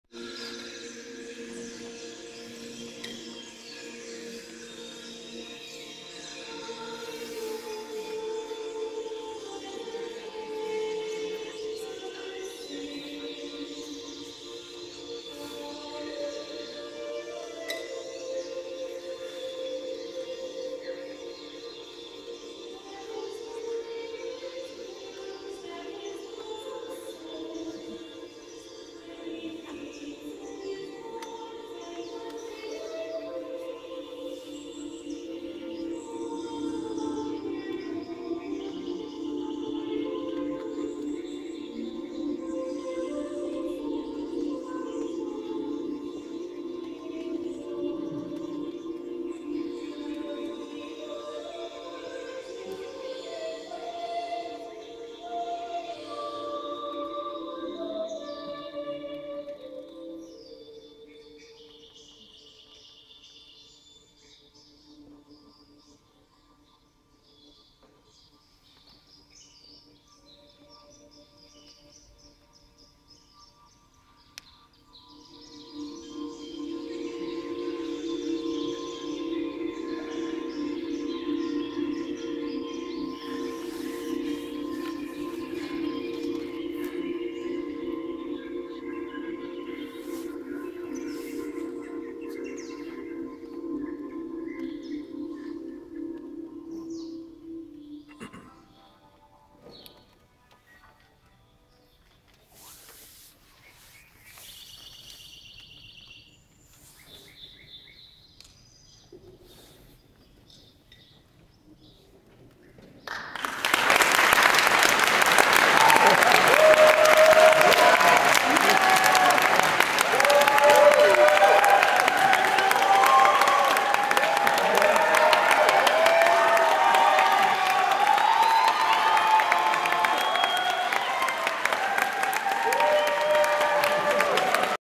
SCU Tweet WP 20150306 003
Santa Clara University Choirs with many smart phones performing at Mission Church